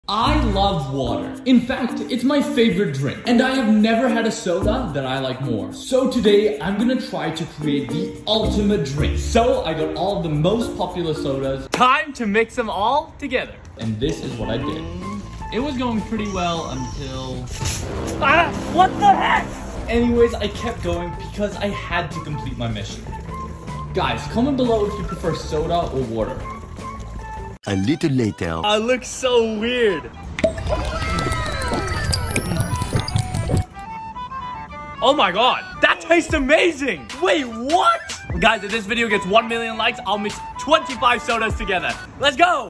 Mixing *10 SODAS* AND DRINKING THEM?!?! (Crazy Result)
Today I combined 10 of the most popular US soda's to make... THE ULTIMATE DIRNK!